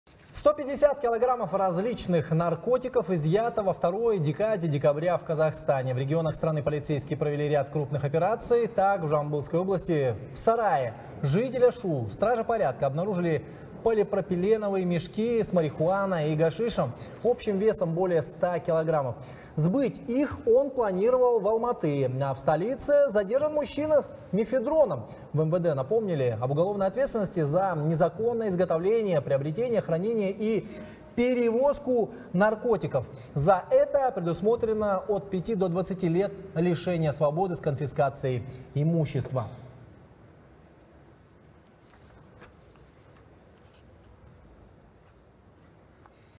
МВД: 150 кг наркотиков изъято за вторую декаду декабря
150 кг различных наркотиков изъято во второй декаде декабря в Казахстане, сообщает «24KZ». В регионах страны полицейские провели ряд крупных операций.